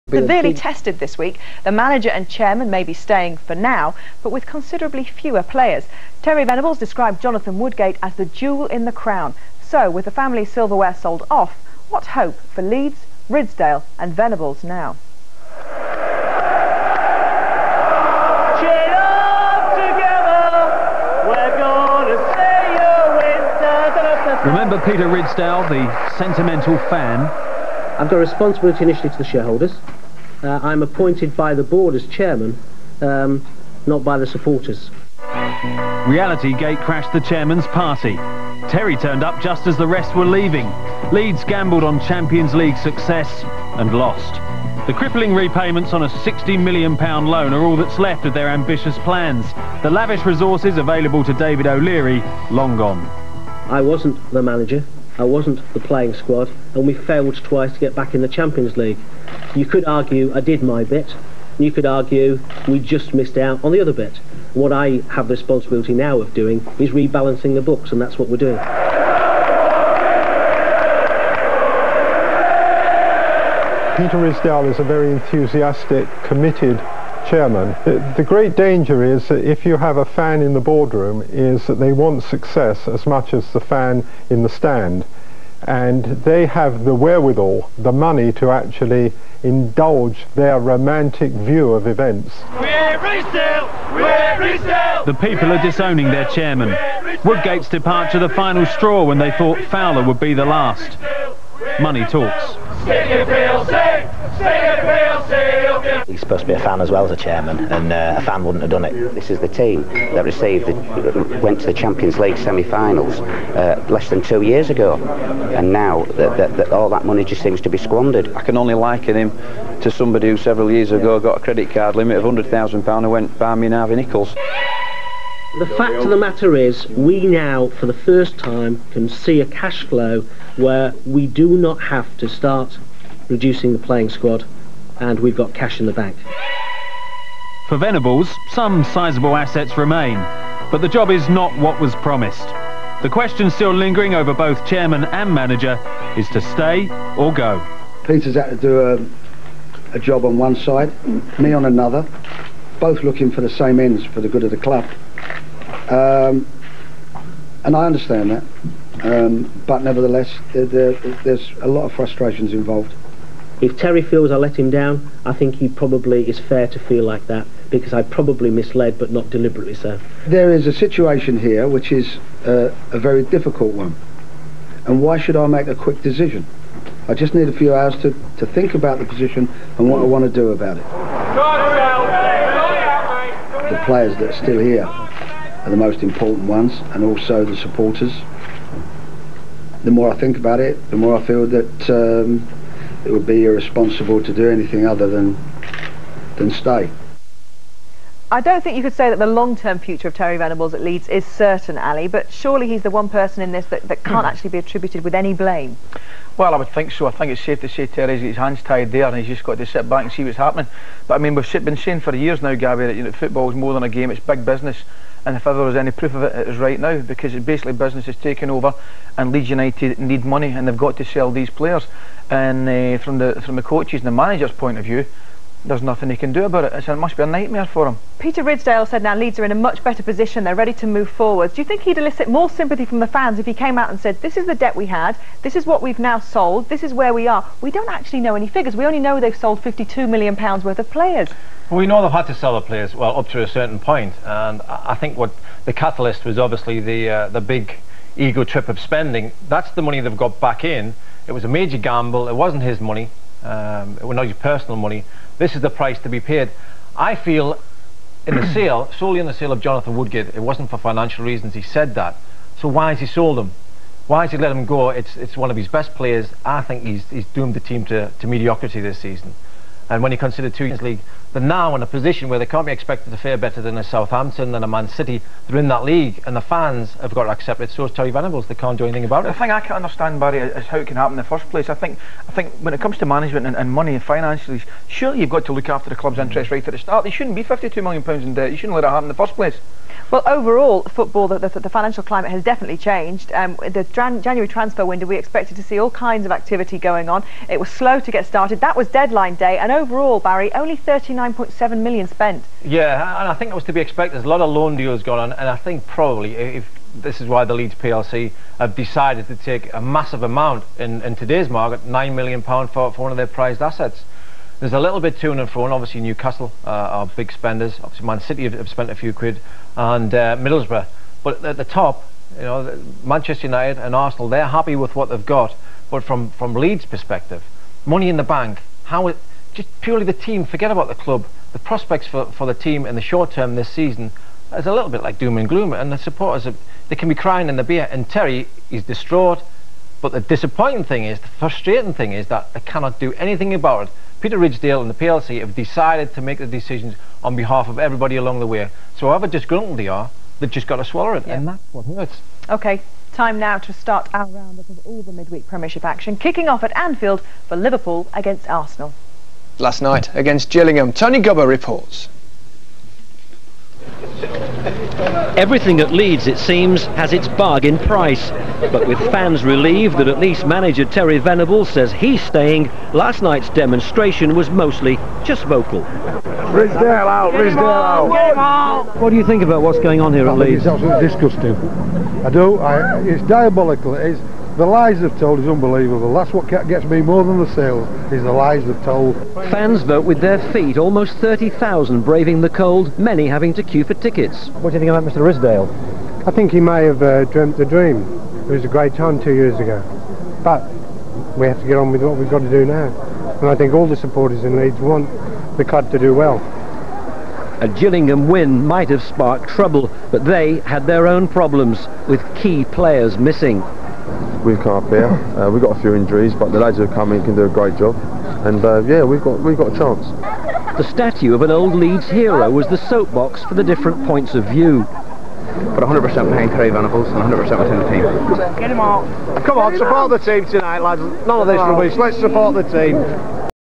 Fallout on the Jonathan Woodgate sale
Report on probably the most shocking sale of all, this really showed how desperate Leeds were. Comments from fans, the manager and the chairman.